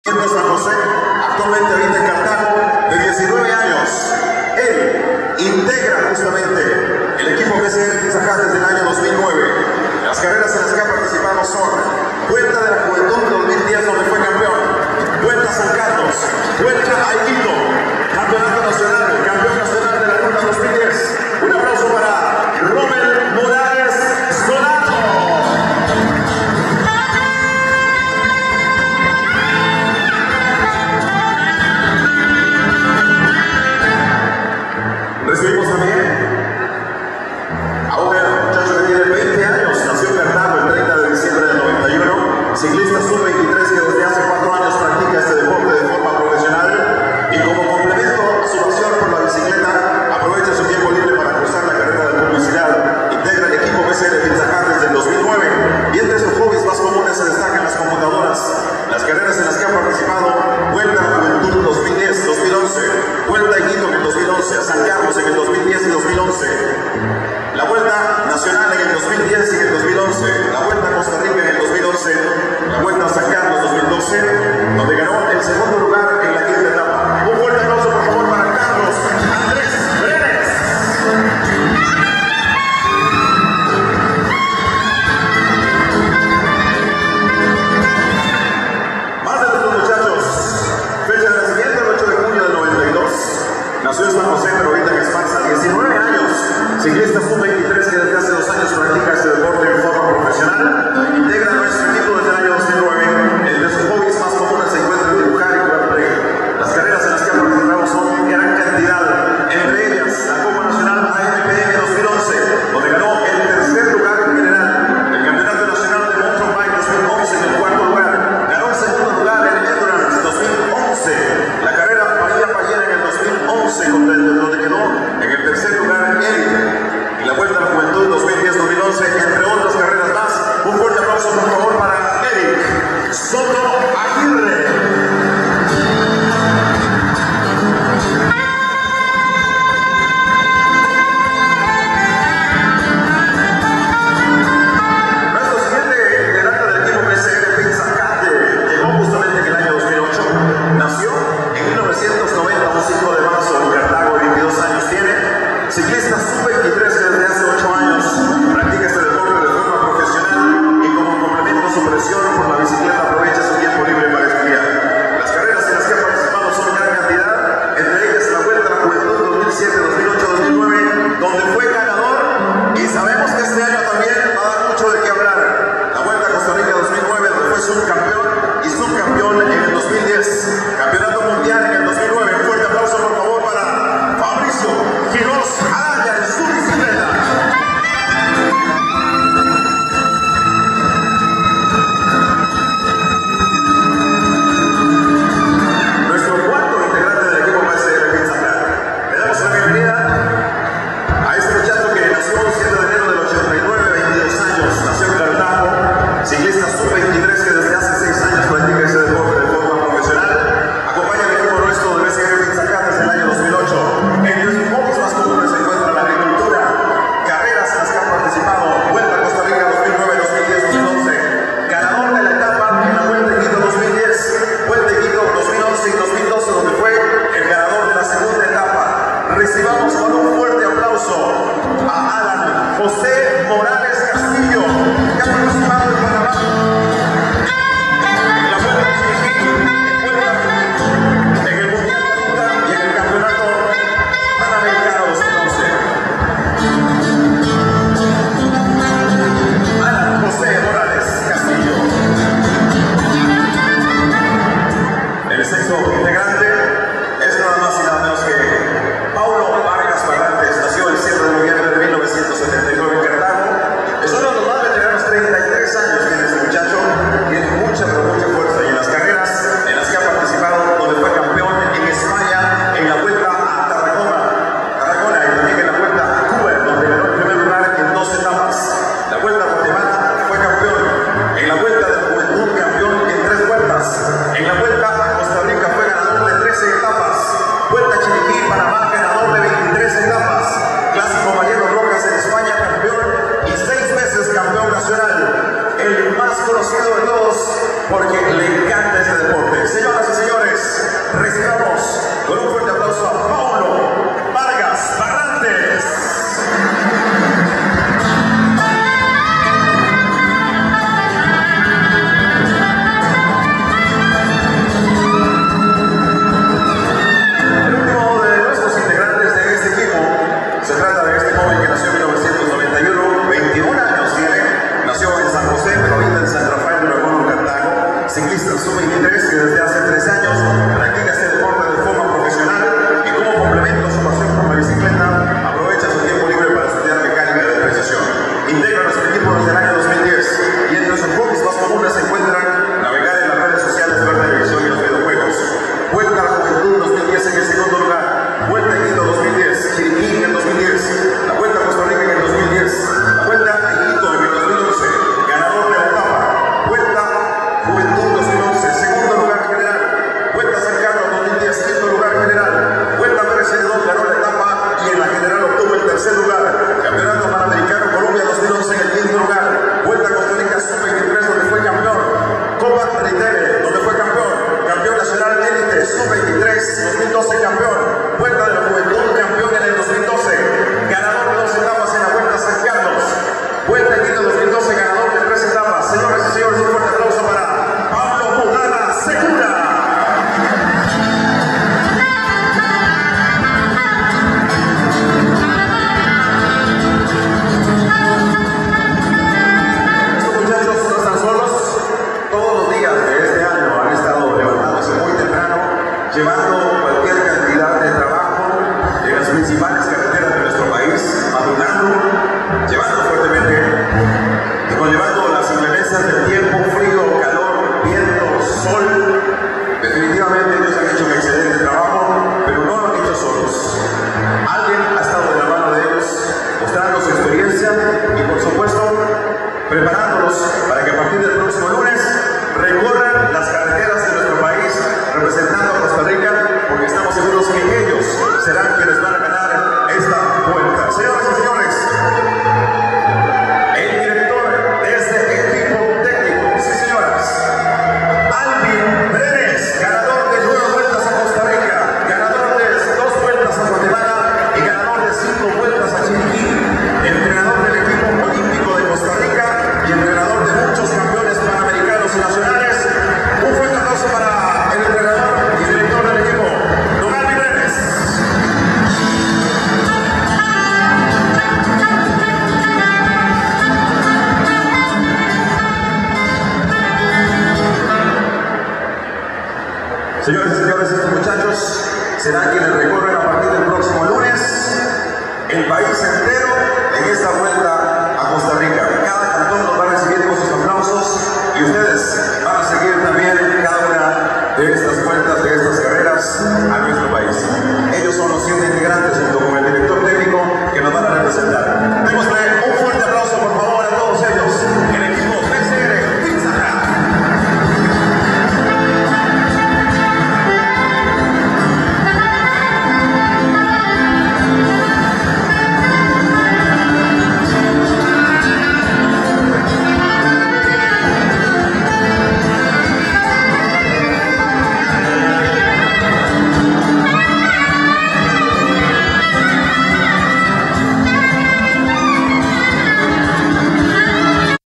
Presentación equipo BCR-PizzaHut que participará en la Vuelta Costa Rica
El equipo BCR-Pizza Hut presentó su equipo de ciclismo para el 2013 y la Vuelta Costa Rica. El equipo está conformado por Erick Soto, Pablo Mudarra, José Quirós, Carlos Brenes, Paulo Vargas, Romel Morales y Alan Morales.